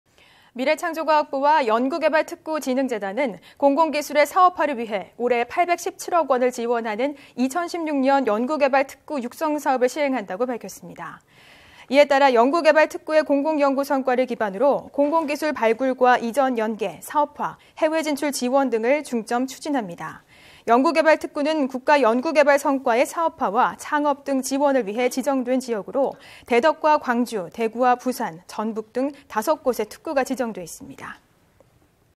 공공 R&D 성과 사업화에 817억 원 지원 / YTN 사이언스
미래창조과학부와 연구개발특구진흥재단은 공공기술의 사업화를 위해 올해 817억 원을 지원하는 '2016년 연구개발특구 육성사업'을 시행한다고 밝혔습니다.

이에 따라 연구개발특구의 공공연구 성과를 기반으로 공공기술 발굴과 이전 연계, 사업화, 해외진출 지원 등을 중점 추진합니다.

연구개발특구는 국가 연구개발(R·D) 성과의 사업화와 창업 등 지원을 위해 지정된 지역으로, 대덕과 광주, 대구와 부산, 전북 등 5곳에 특구가 지정돼 있습니다.